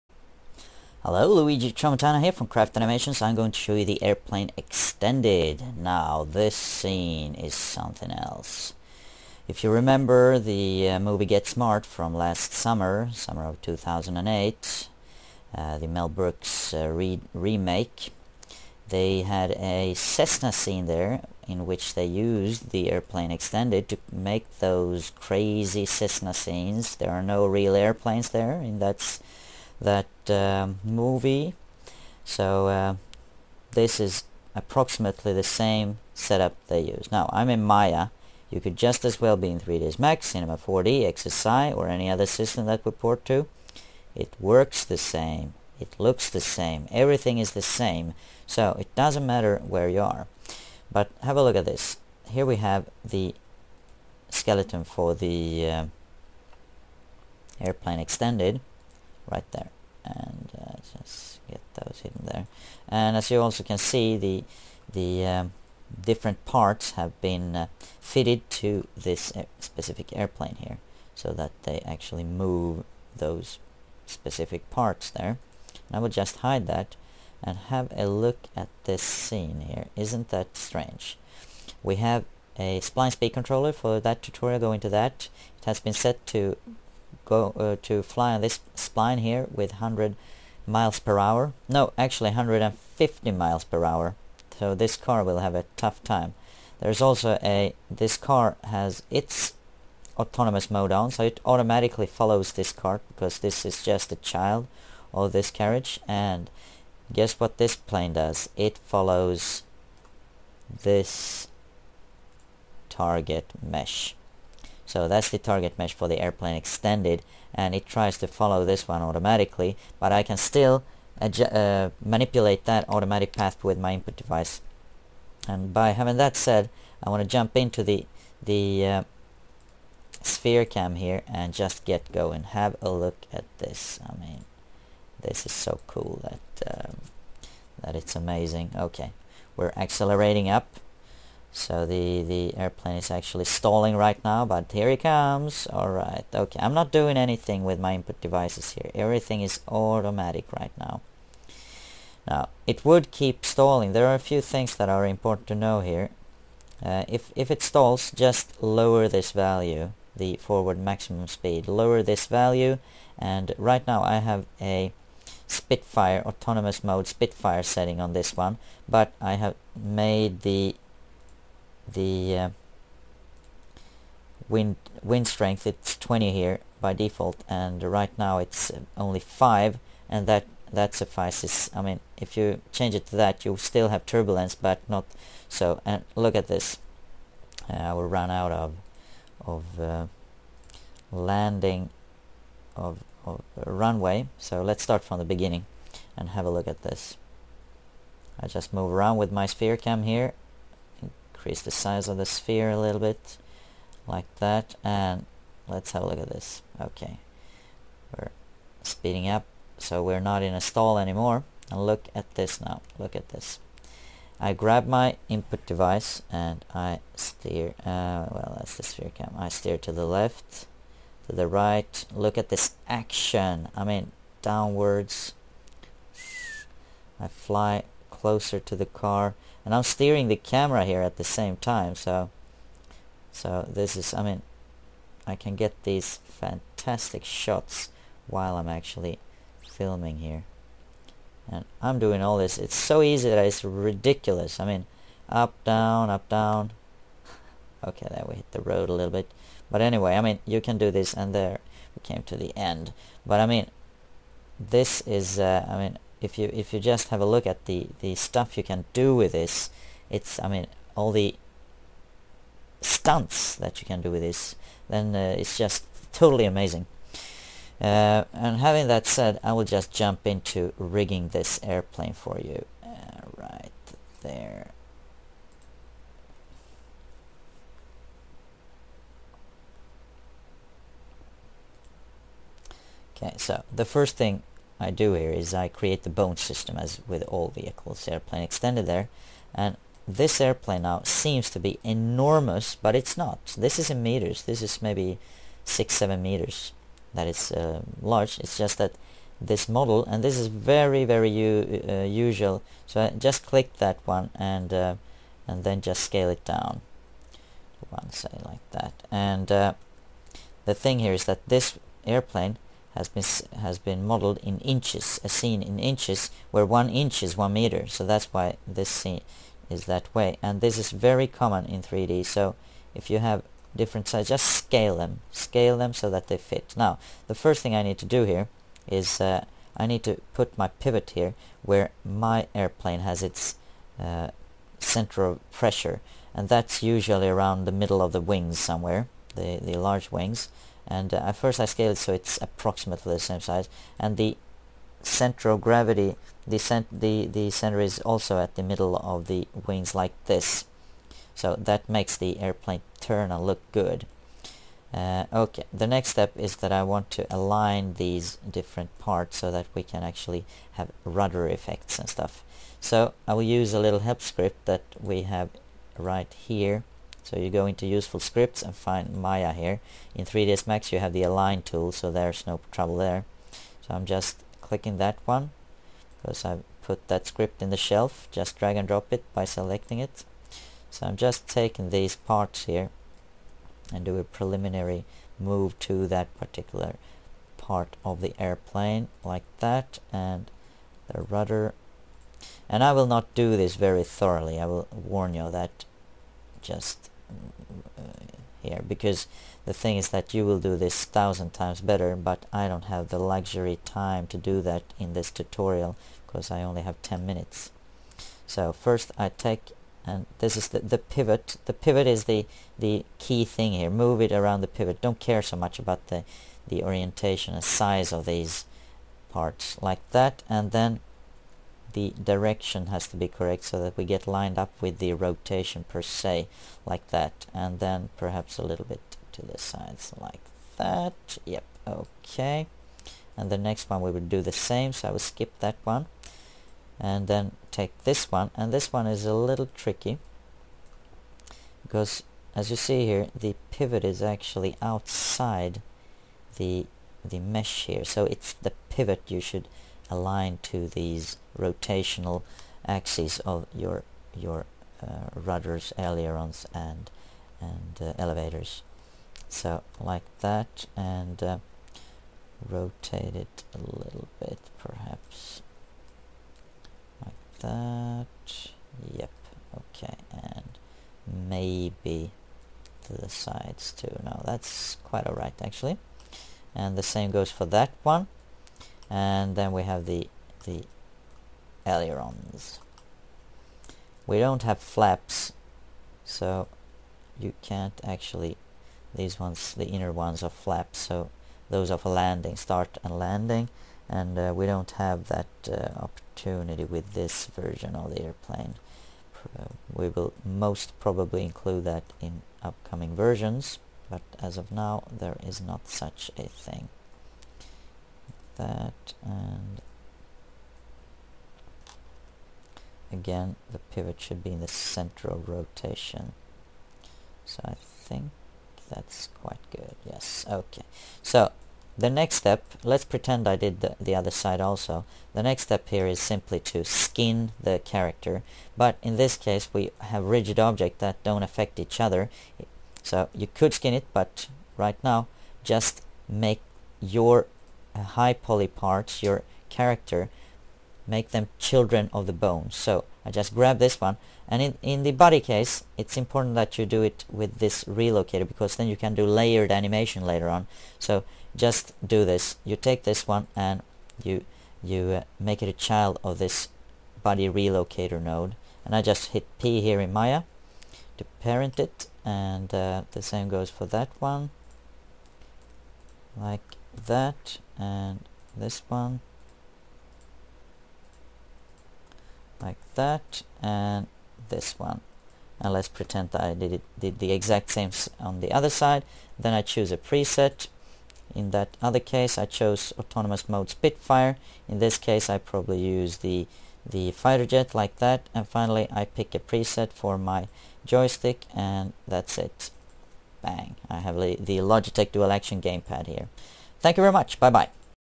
How to rig and animate aircraft using Airplane Extended - Craft Director Studio in 3ds Max & Maya
In this turbo tutorial on Craft Airplane Extended you will learn how to use the "autonomous mode" (the autopilot) to create the stunning action scenes like Digital Dimension did for the movie GET SMART (2008) where they created the crazy cessna flight scenes with this tool. Also learn how to rig a fighter jet inside of 3ds max or Maya, all with Craft Director Studio.